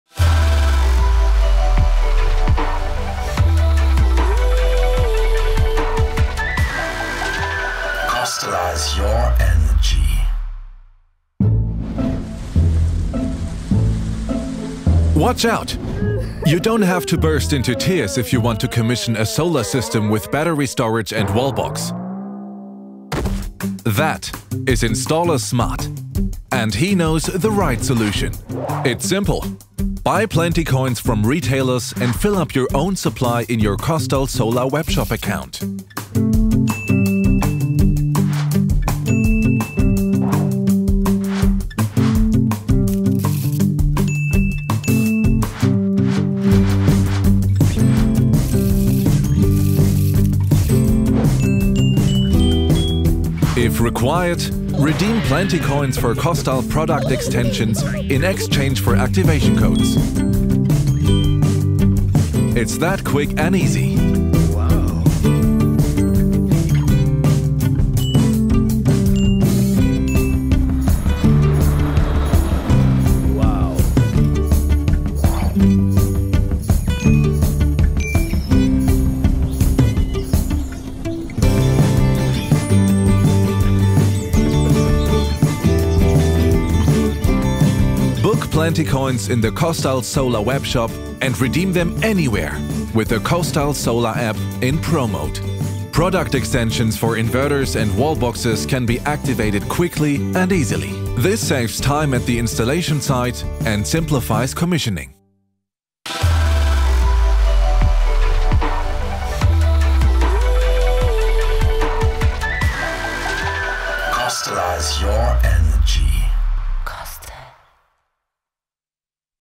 InstallerSmart: PLENTICOIN – the new single-purpose voucher for KOSTAL product upgrades
NEW: PLENTICOIN – the new single-purpose voucher for KOSTAL product upgrades
Use it digitally or as a card: the new KOSTAL PLENTICOIN makes things easier, faster and more flexible.

► What is the KOSTAL PLENTICOIN?

The PLENTICOIN is a single-purpose voucher for all KOSTAL product extensions. When installing the device, the PLENTICOIN can be redeemed in the KOSTAL Solar Webshop for the required activation code. The PLENTICOIN is purchased through specialist wholesalers.

The PLENTICOIN comes in two forms: an "analogue" version in the form of a sealed voucher card and a "digital" version for drop shipment business.

As a voucher card, the PLENTICOIN is available in three versions for the following regions: Germany, EU (excluding Germany) and non-EU. For tax reasons, the PLENTICOIN card can only be redeemed in the relevant country.

Please note: Product upgrades using an activation code change the device parameters, which impact how the installation is carried out. Therefore, only installers are permitted to redeem PLENTICOINs for product upgrades.

► What product upgrades are available?

• Battery upgrade
• Power upgrade
• Comfort function

Let’s connect!